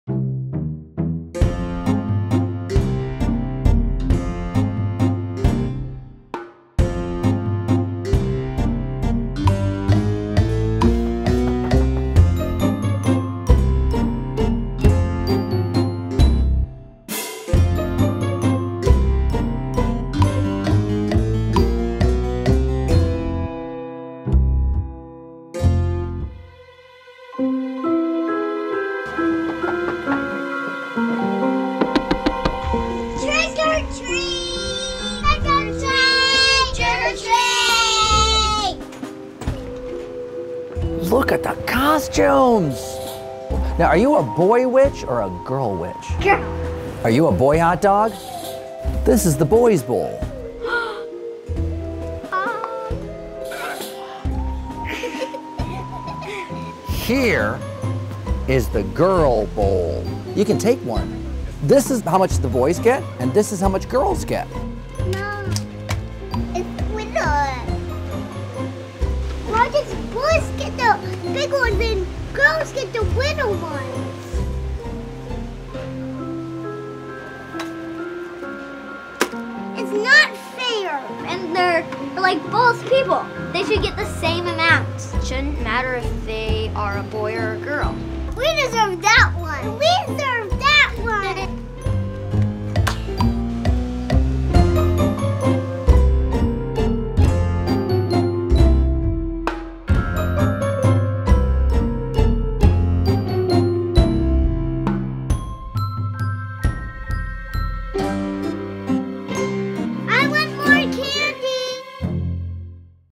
The Wage Gap: A Scary Truth
What if there were a gender wage gap for trick or treaters? Share and help us build a world where these little girls never have to deal with the real thing.